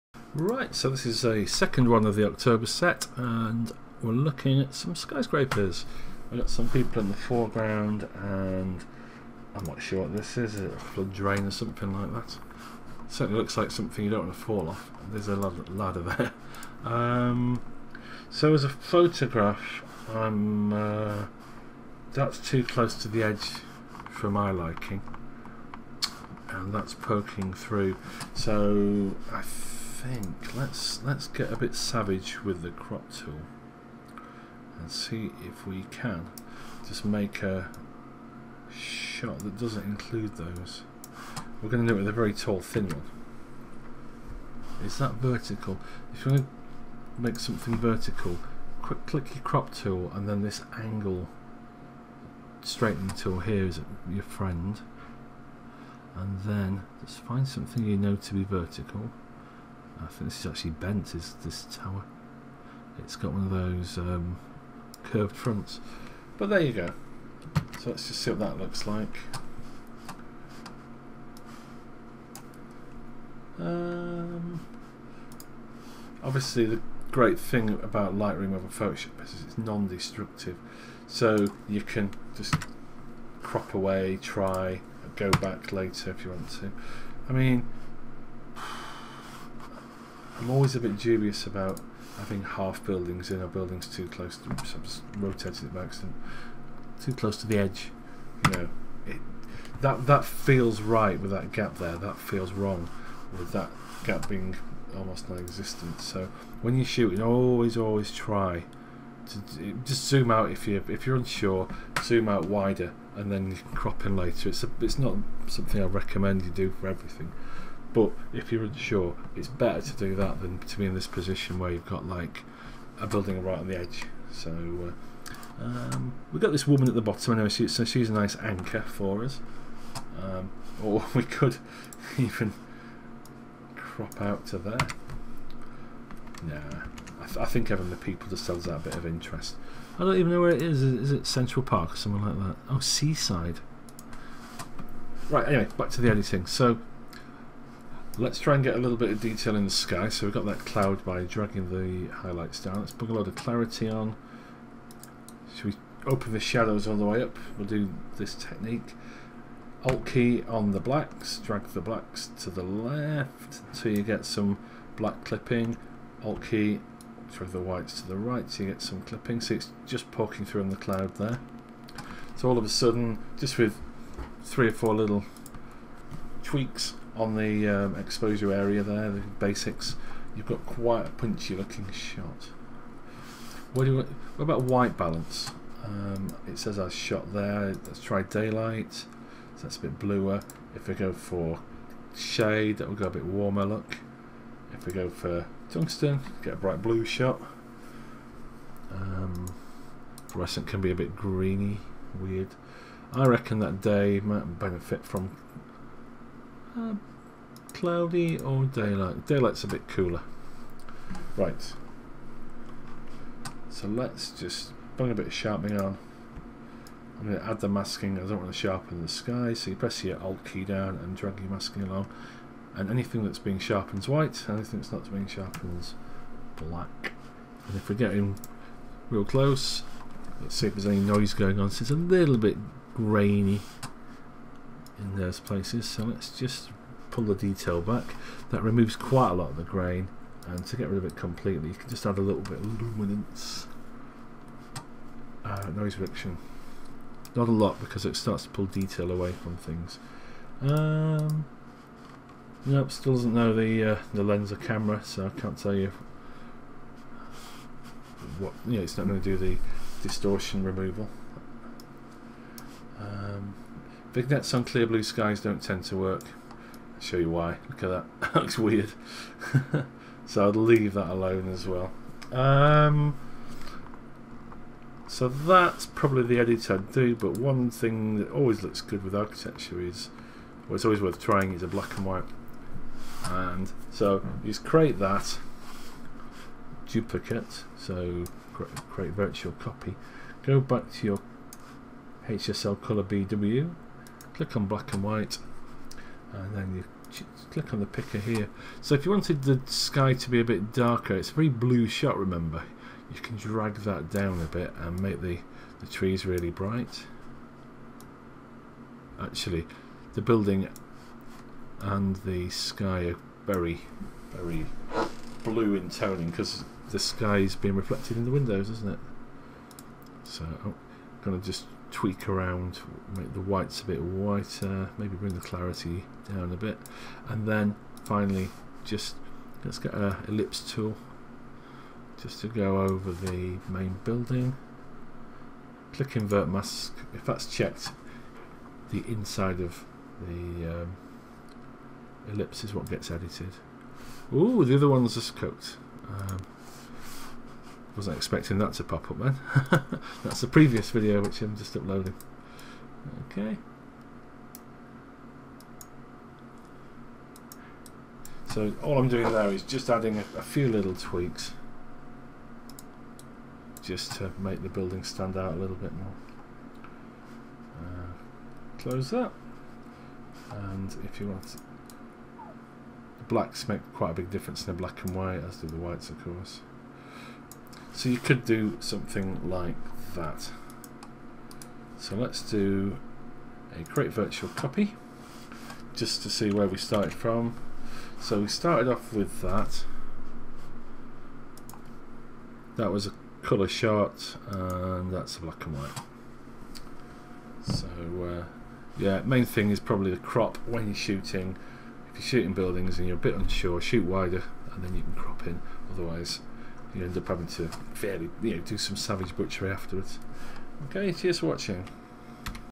Lightroom free tutorial - Skyscraper
Lightroom Free help - this one is a cityscape - the scene has 2 buildings - one which I just didn't feel sat right, too close to the edge.

I show a few different crops, then do some basics to make the building punchier - and end on a black and white conversion